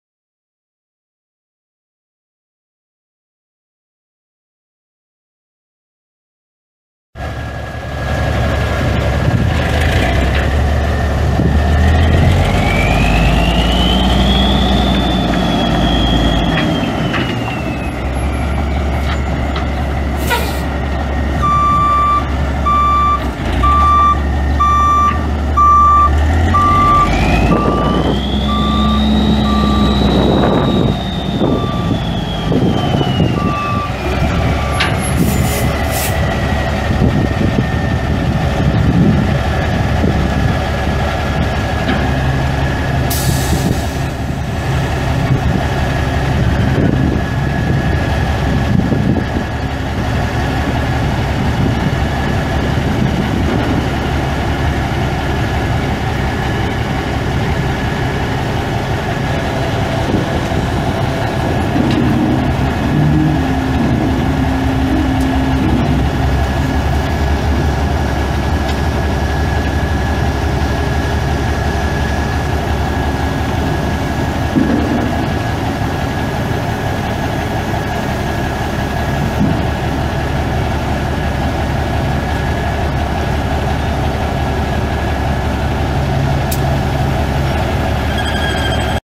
1988 International F8300 dump truck for sale | no-reserve Internet auction June 22, 2017
1988 International F8300 dump truck
729,505 miles on odometer
Cummins 14.0L L6 turbo diesel engine
350 HP
Eaton Fuller 12 speed manual transmission
Tandem axle
Inter-axle differential lock
PTO
Heat
AM/cassette
Heated mirrors
Air suspension seat
Heil HPD2YD15 steel dump bed
15'L x 8'W
42" side height
8" side board height
Pintle hitch
Trailer wiring
196" wheelbase
Spring suspension
Air brakes
Engine brake
11R24.5 tires
Engine oil leaks
Missouri title